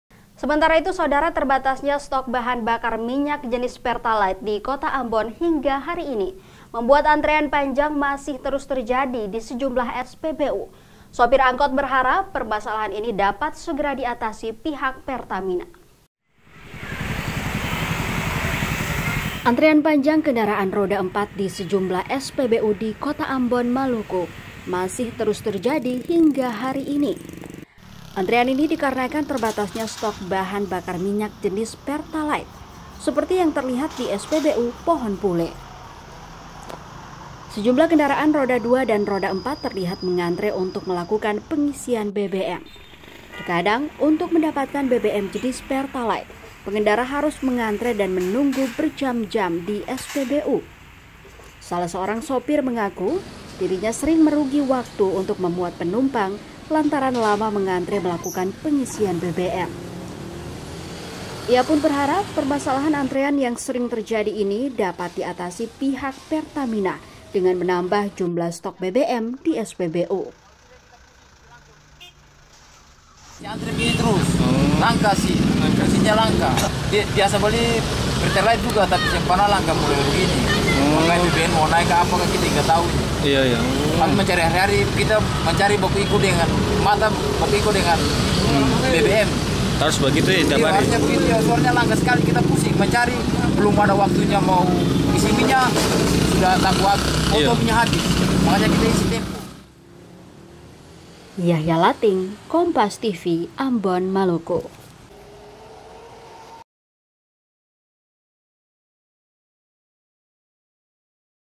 Antrian BBM Masih Terus Terjadi di Sejumlah SPBU
AMBON,KOMPAS.TV-Antrian panjang kendaraan roda empat di sejumlah spbu di Kota Ambon Maluku masih terus terjadi hingga hari ini. Antrian ini dikarenakan terbatasnya stok bahan bakar minyak jenis pertalite.

Seperti yang terlihat di spbu Pohon Pule, sejumlah kendaraan roda empat dan roda dua terlihat mengantri untuk melakukan pengisian bbm, terkadang untuk mendapatkan bbm jenis pertalite pengendara harus mengantri dan menunggu berjam-jam di SPBU.

Salah seorang sopir mengaku dirinya sering merugi waktu untuk narik penumpang lantaran lama mengantri melakukan pengisian bbm, dirinya berharap agar permasalahan antrian yang sering terjadi ini dapat diatasi pihak pertamina dengan menambah jumlah stok BBM di SPBU.